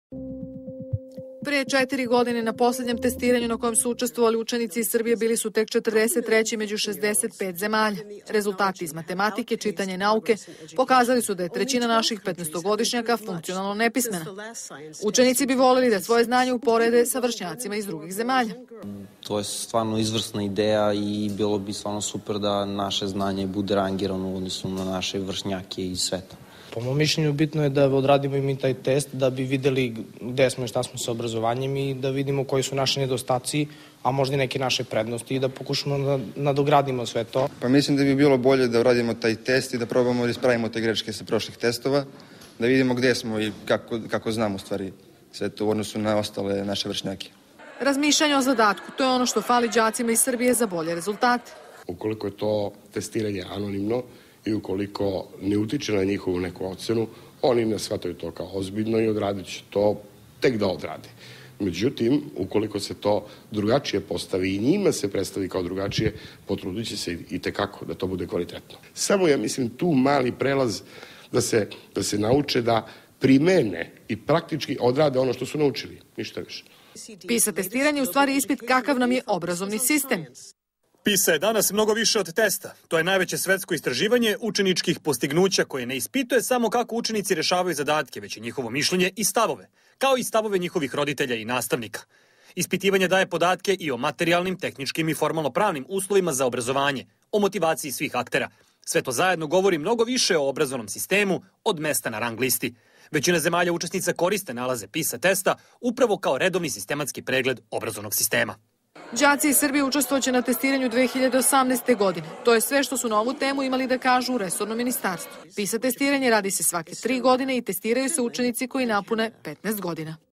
PISA lista ove godine bez Srbije
PISA testiranje, najveća međunarodna provera učenika, prošla je ovog puta bez učešća učenika iz Srbije. Razlog izostanka je kašnjenje u procedurama. Najbolje rezultate, na testiranju urađenom prošle godine, koje je sada objavljeno, imali su Singapur, Hongkong i Japan.